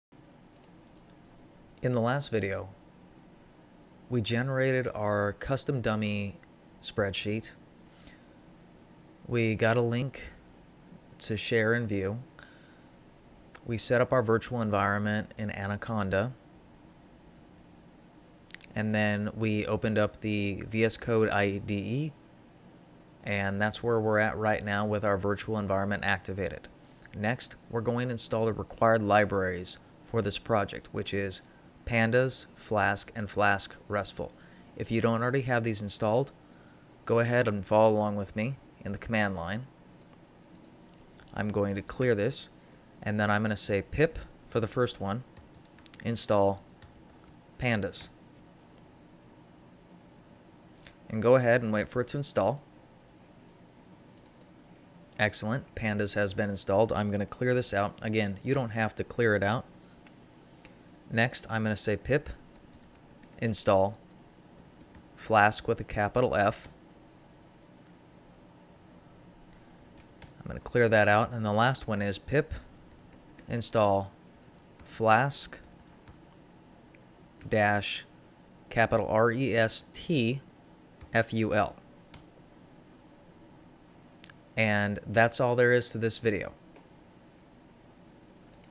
Simple Python Flask API with Google Sheets - 02 - Installing the Required Libraries
Install the required python libraries for the Simple Python Flask API with Google Sheets application. This is the second video in the tutorial.